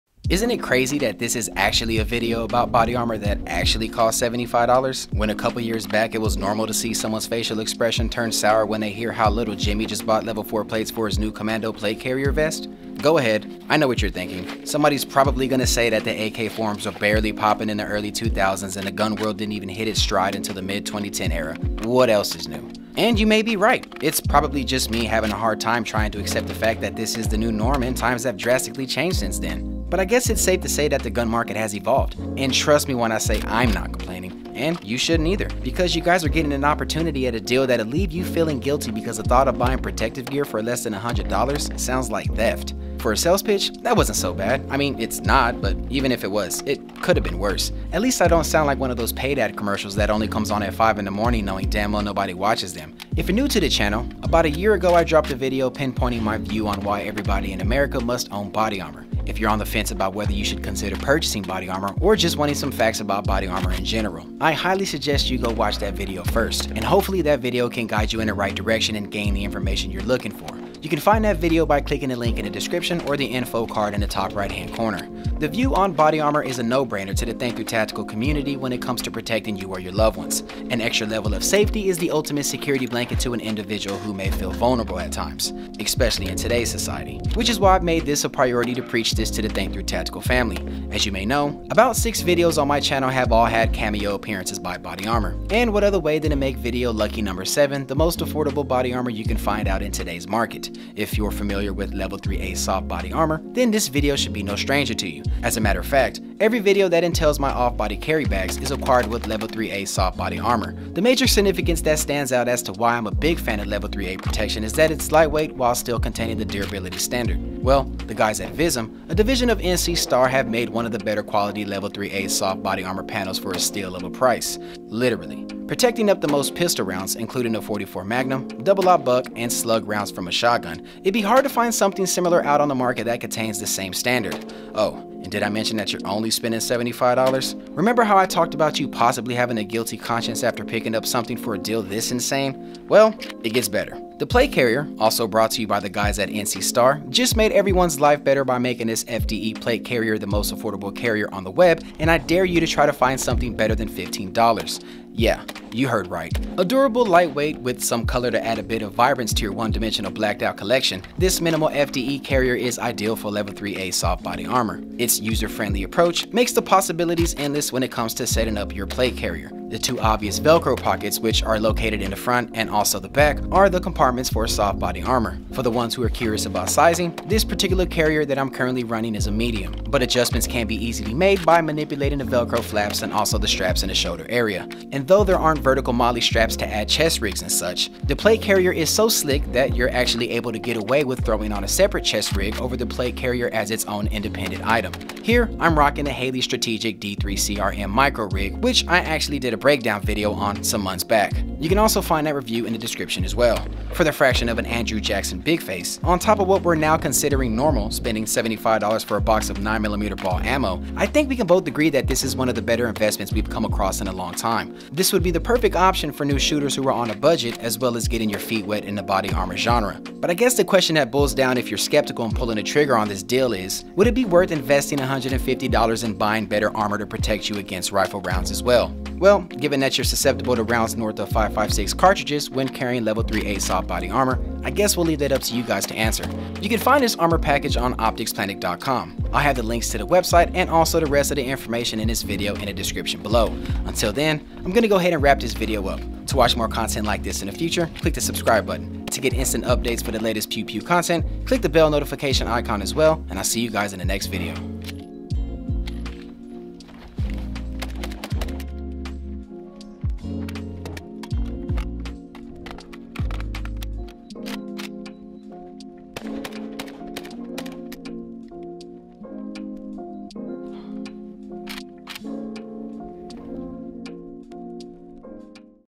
Introduction To Body Armor: Why This $75 Soft Armor + Plate Carrier Is Perfect For New Shooters
We all know I’m a sucker for deals and I found this stupid cheap bargain on this $75 Level IIIA Soft Body Armor that might interest some of you who are new to the body armor community. If you’re a new shooter, watch this!!